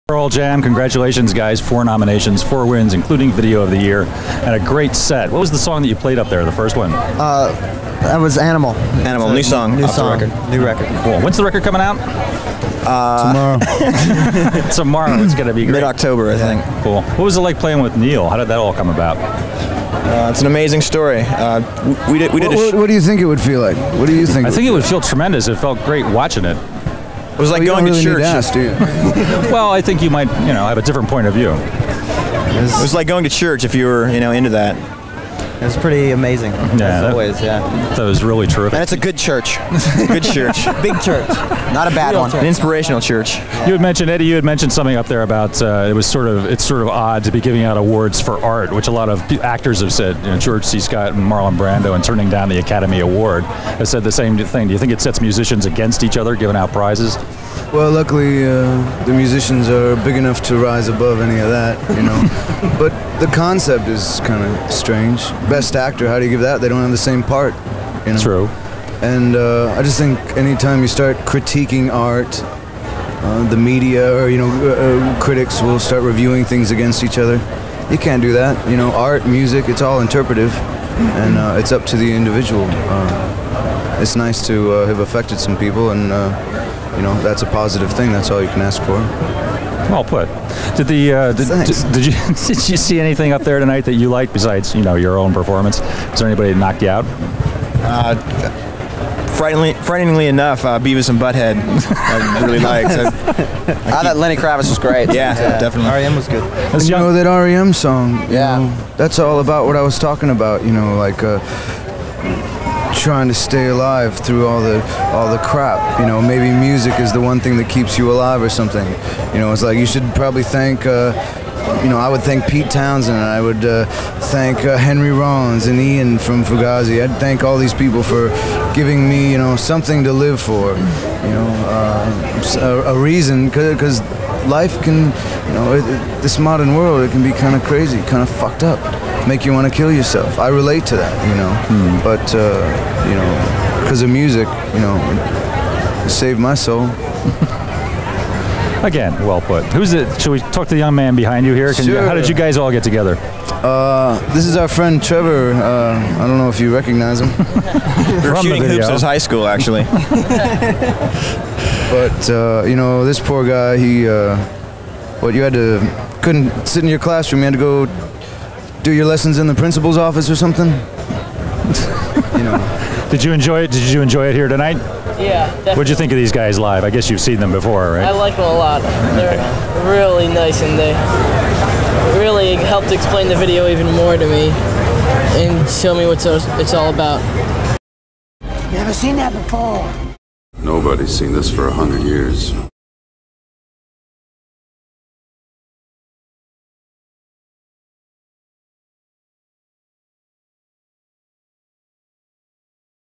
Pearl Jam Interviewed at the 1993 MTV Music Awards
Pearl Jam Interviewed at the 1993 MTV Music Awards after winning several awards.  I don't remember, but this must have been live since Eddie Vedder drops an f bomb that isn't bleeped.